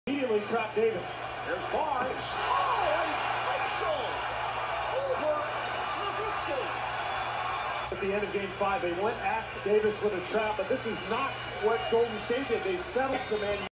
Matt Barnes goes lefty on Dirk Nowitzki with a facial - 2007 Western Conference Round 1 Game 6
Matt Barnes beats the trap and raises up over Dirk Nowitzki for a left handed dunk. (2007 Playoffs- Game 6 Warriors vs Mavericks).